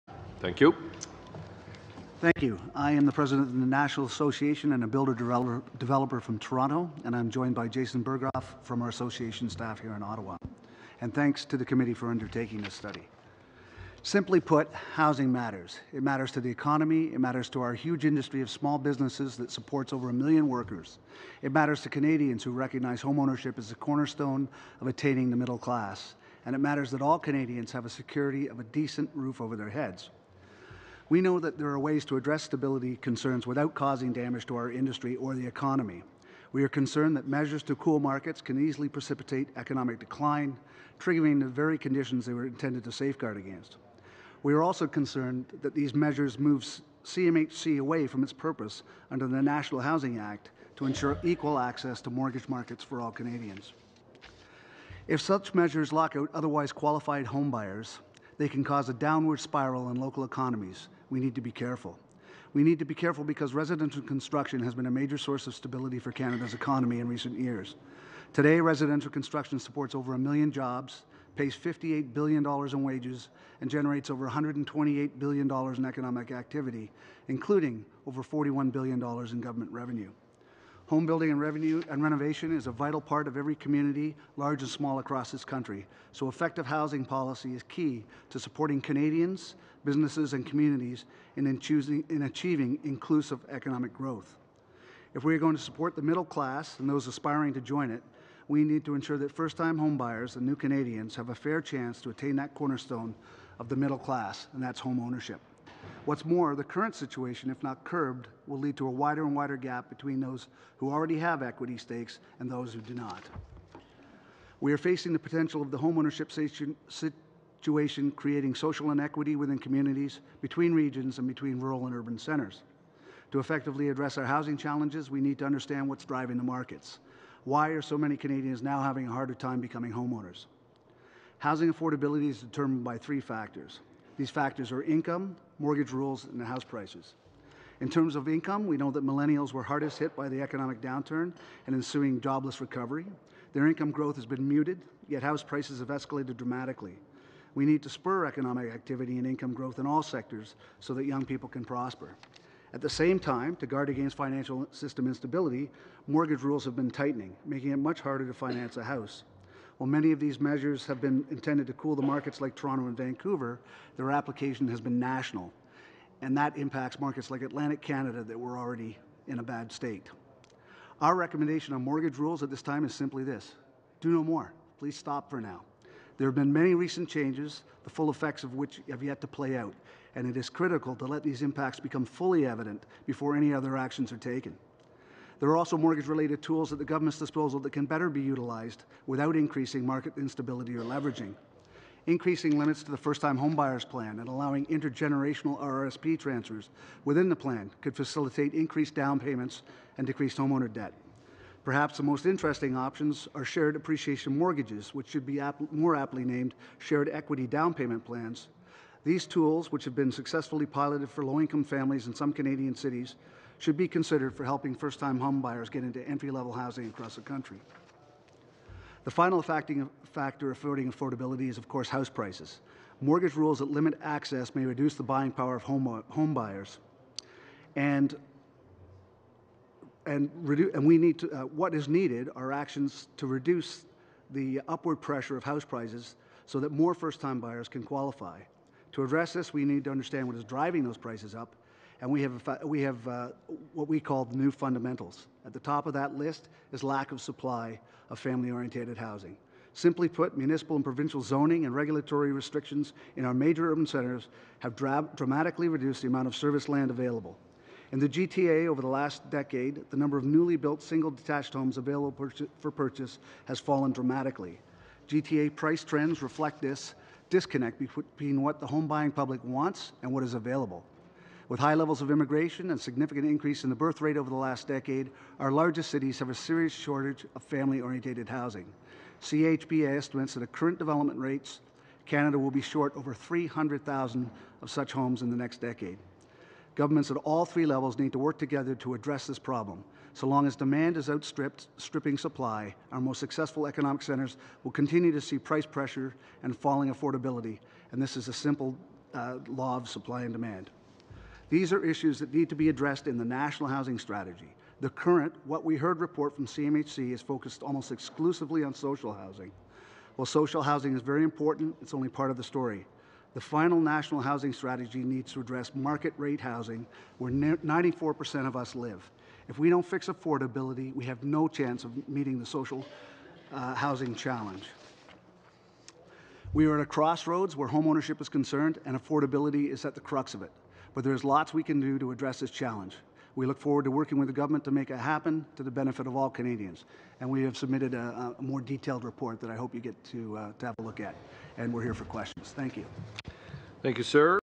CHBA Remarks to FINA - February 1, 2017
Canadian Home Builders' Association President Bob Finnigan's remarks to the Finance Committee from Feb 1, 2017, regarding issues surrounding the real estate market; the potential impact of the housing market on the financial system and challenges surrounding access to home ownership.